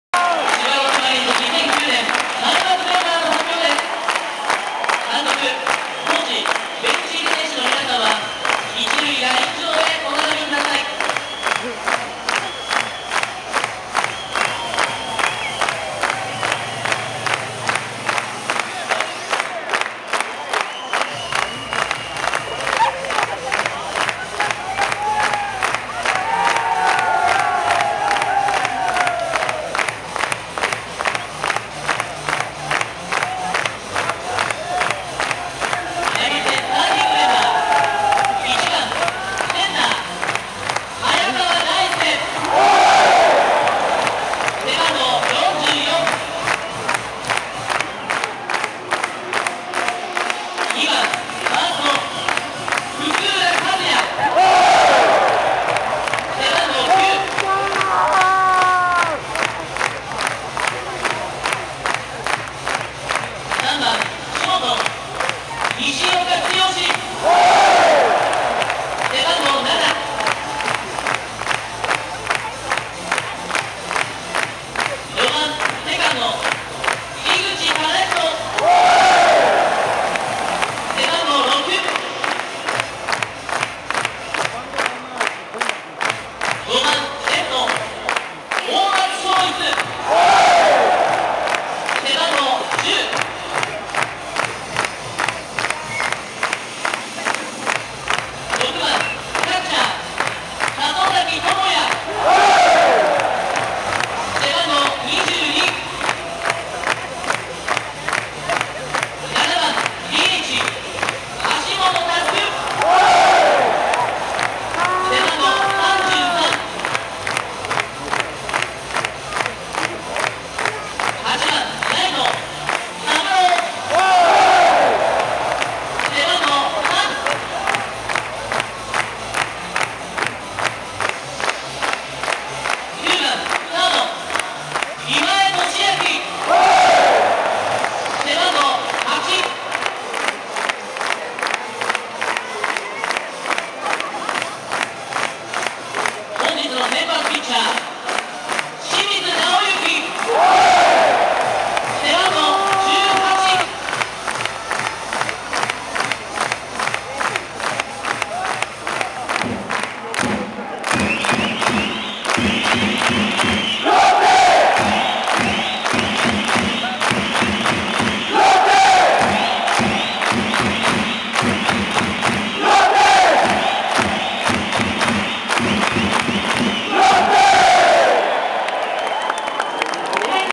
スタメン(千葉ロッテマリーンズ)
2009年4月3日 第1回戦 埼玉西武ライオンズ対千葉ロッテマリーンズ　千葉マリンスタジアム

1(中)早川大輔
2(一)福浦和也
3(遊)西岡剛
4(二)井口資仁
5(左)大松尚逸
6(捕)里崎智也
7(指)橋本将
8(右)サブロー
9(三)今江敏晃
P(投)清水直行